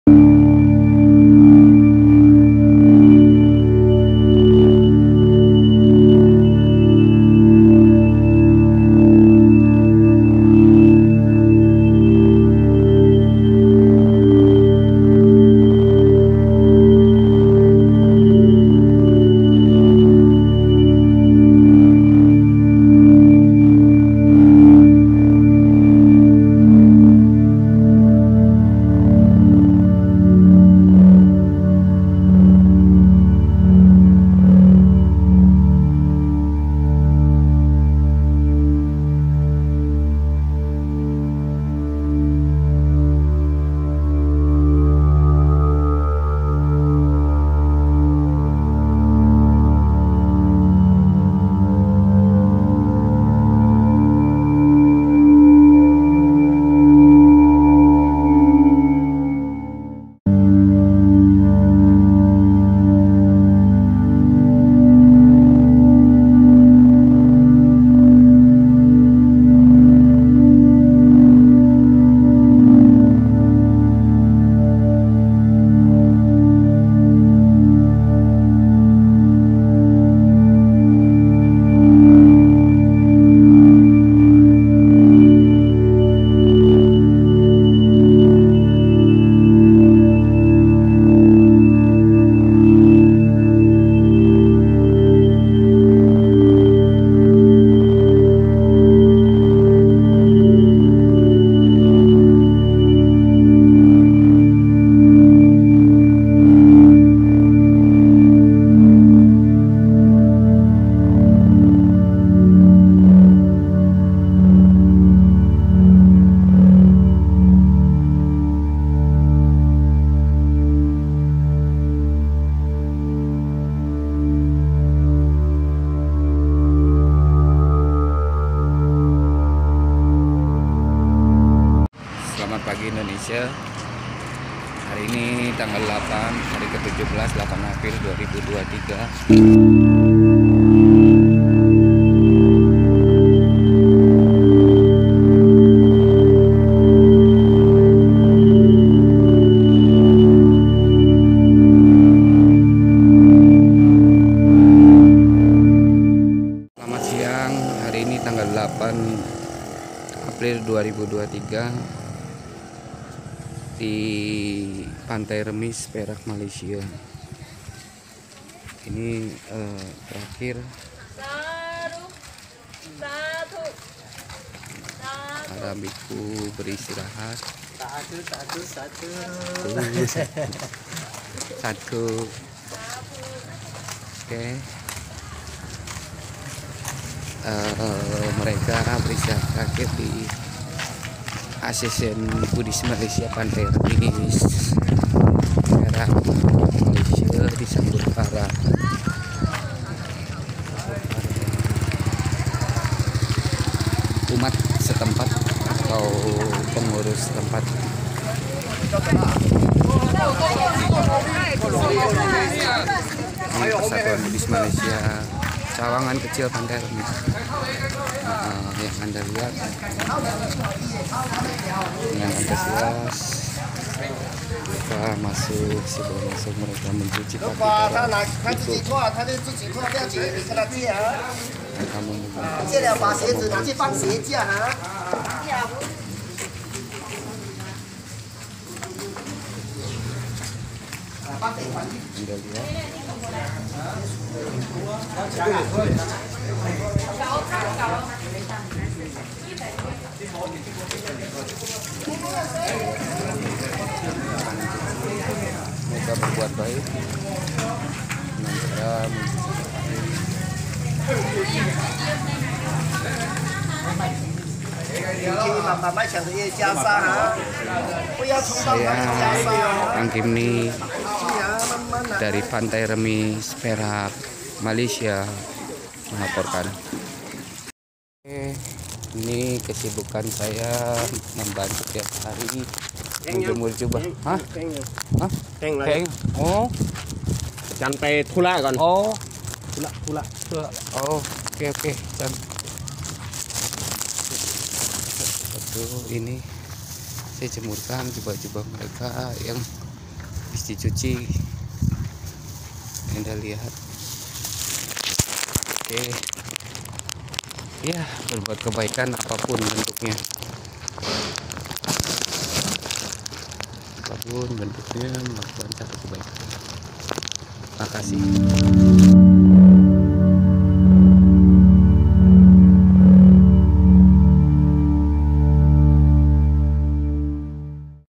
Thudong. Perjalanan Hari Ketujuhbelas
Thudong memasuki hari ke tujuh belas. berangkat dari sungai tingi dan beristirahat di pantai remis perak

Thudong adalah perjalanan spiritual para bhante bikhu dengan berjalan kaki dari Thailand, Malaysia, Singapura, dan berakhir di Candi Borobudur Indonesia. Di ikuti oleh 31 Bhante dan 2 umat.

Semoga semua makhluk di bumi berbahagia.

@srabad chanel
shui suryawan
waisak